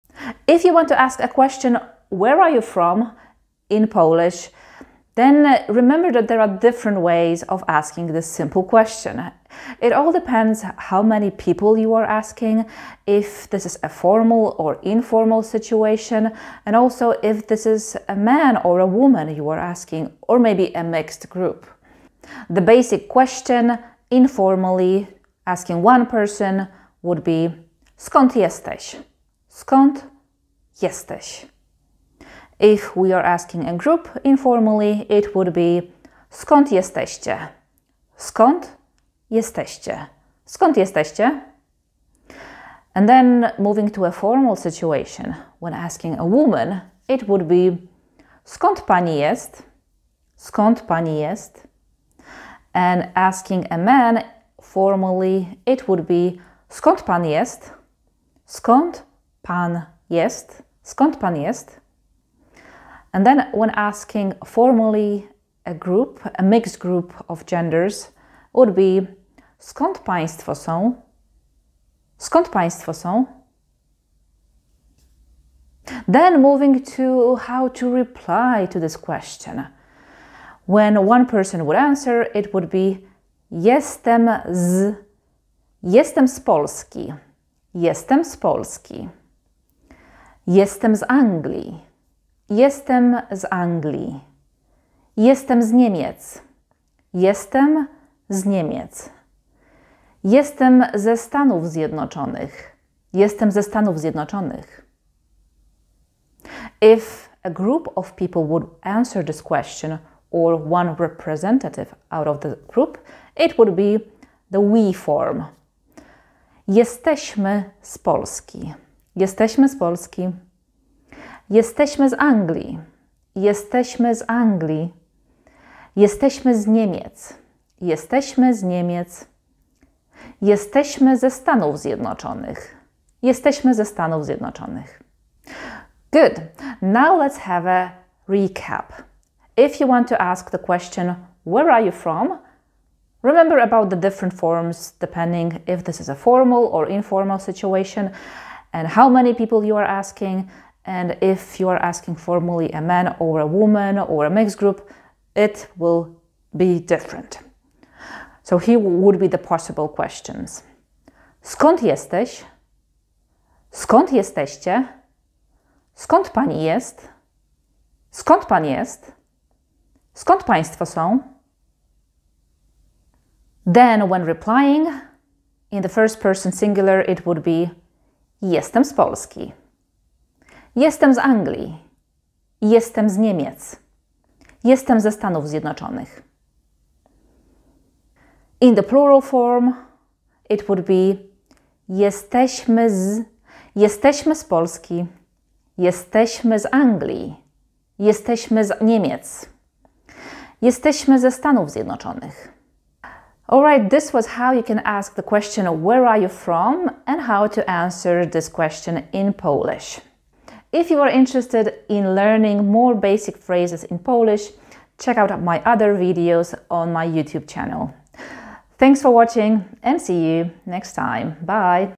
'Where are you from?' in Polish
In this lesson you will learn how to ask the question 'Where are you from?' in Polish and how to reply to it.